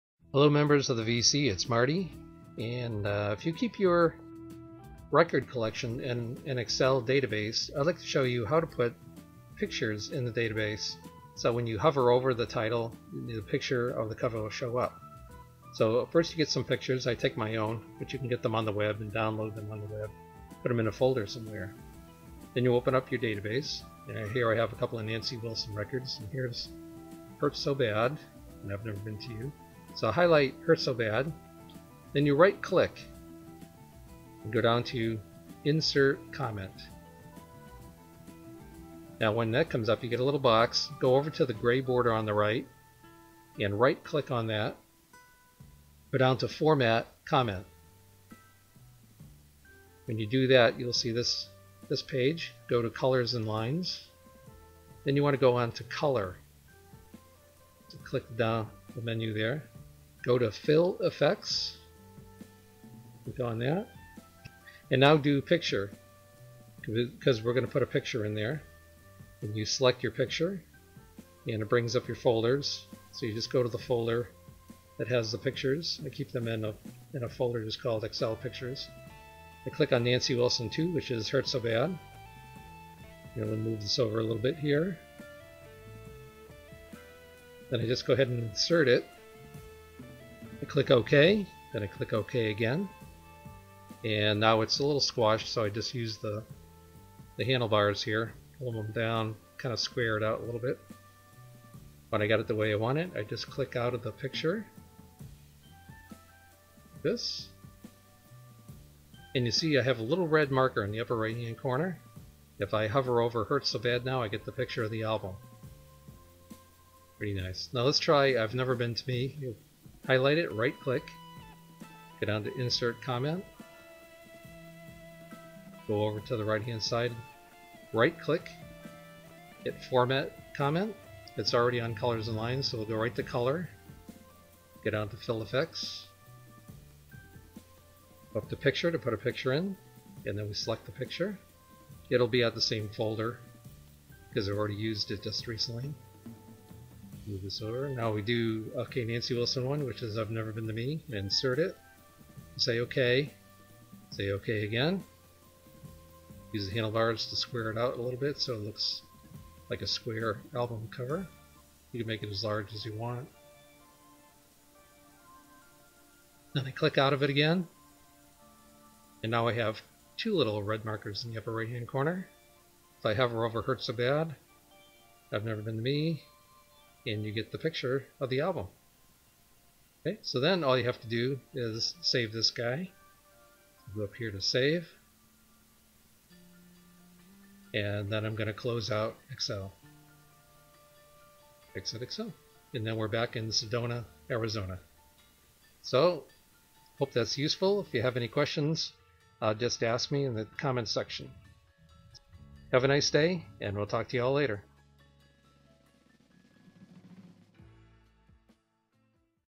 Excel Record Album Database - Add Cover Art
Some of you keep track of your albums in an excel spreadsheet.  Here's how to add the album cover to your database.
I was unable to find an excel function that creates a default size for the picture (meaning 'comment').   I could have written a quick macro to do that for me, but someone already did, so I decided not to re-invent the wheel.
Here is the macro:

  Sub AdjustComments()  (new line)

Dim c As Range, r As Range  (new line)

     Set r = Range("B1:B1200") 'adjust range reference as needed  (new line)

      Application.ScreenUpdating = False  (new line)

     On Error Resume Next  (new line)

     Application.ScreenUpdating = True  (new line)

End Sub

Pretty simple.  The range is the column.  Currently at 1200 but when I have more albums than 1200 I'll edit and increase that.  The '200' references are pixel size, so all my photos are changed to 200 pixels square.
After I add some pictures to the album titles, I just run the macro and it auto-magically updates them all to 200 pixels square.
The description jams my text together, so I added (new line) just to show you to go to a new line.